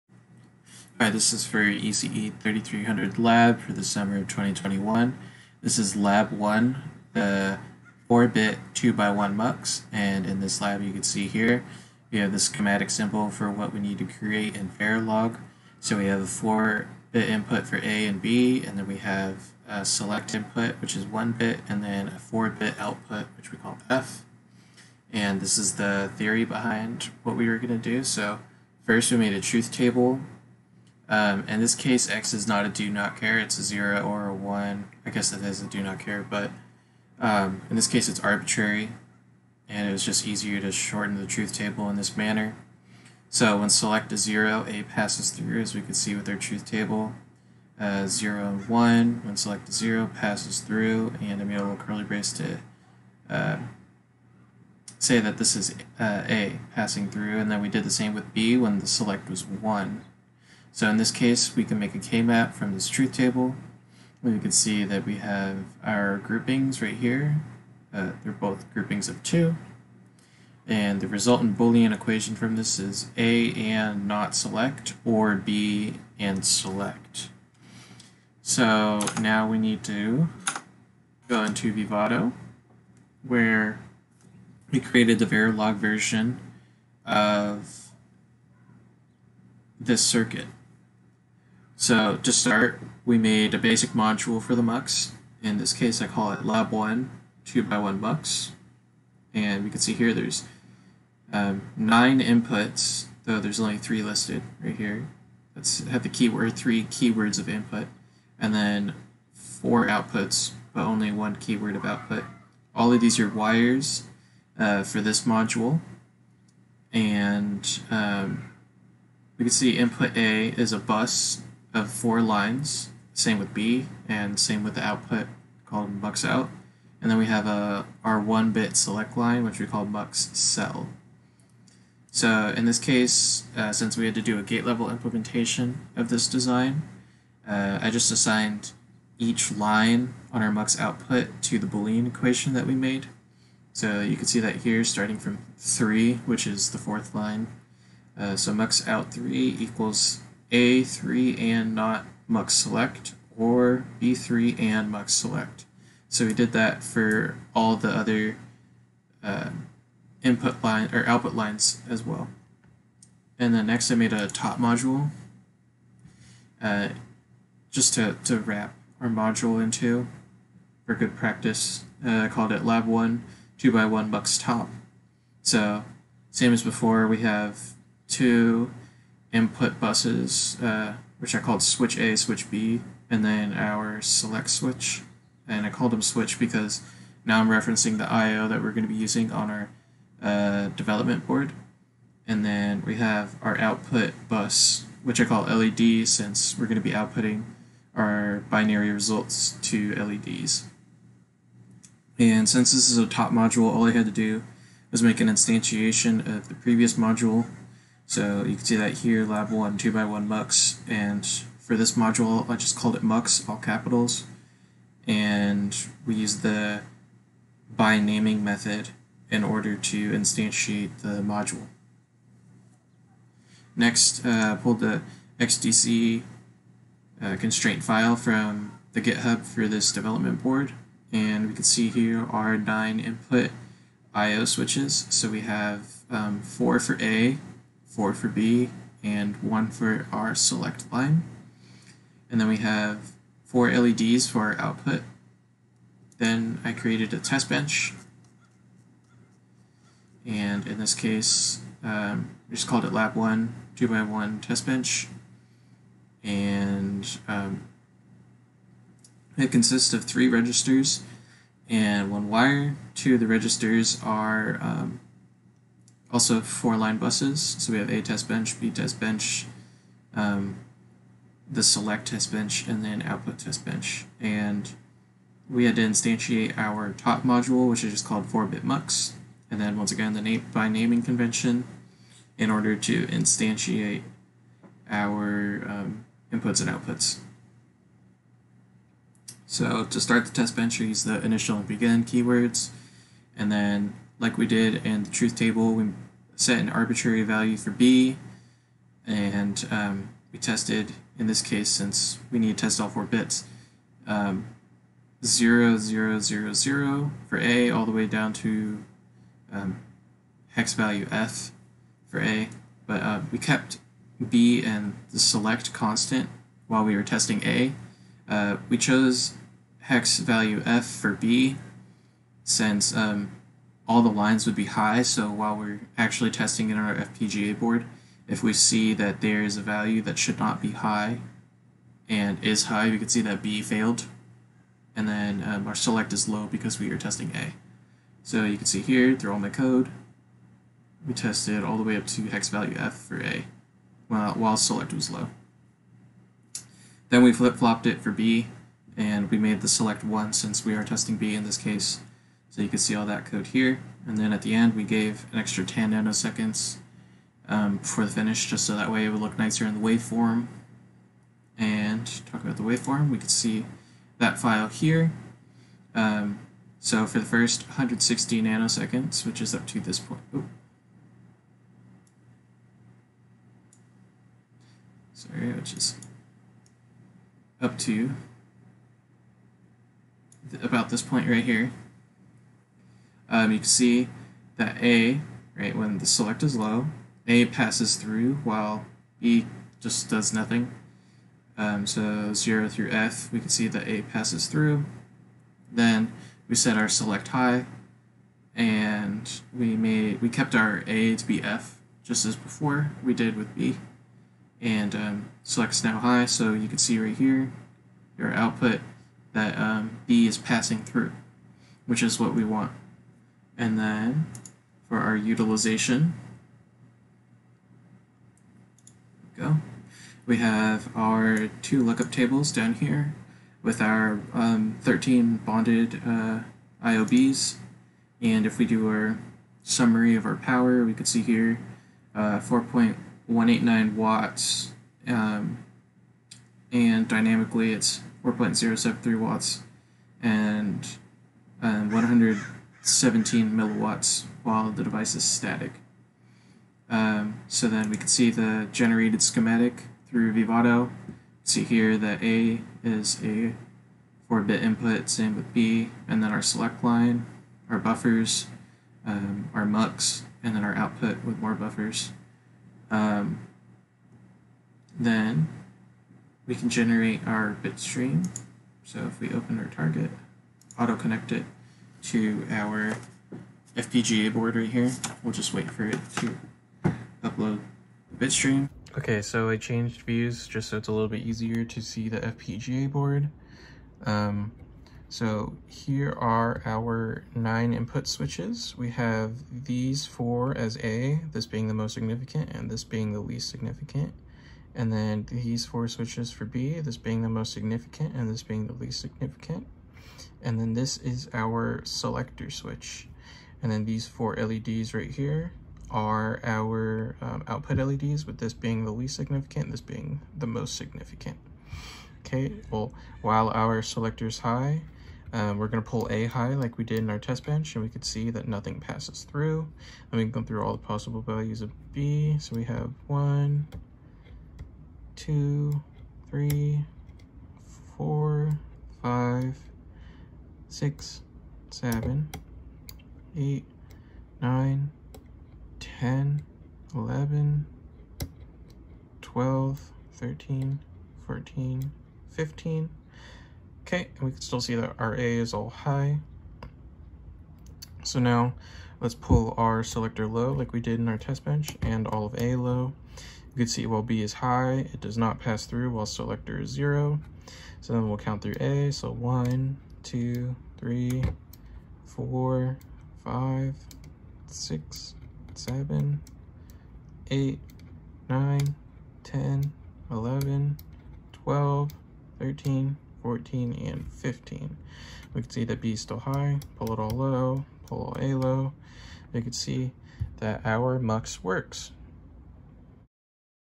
Lab 1 - 4-bit 2x1 Mux
Cal Poly Pomona ECE department
ECE 3300L - Digital Circuit Design Using Verilog
Summer 2021
Professor Mohamed Aly

Hardware: Nexys4 DDR Artix-7 100T FPGA development board
Software: Vivado 2019.2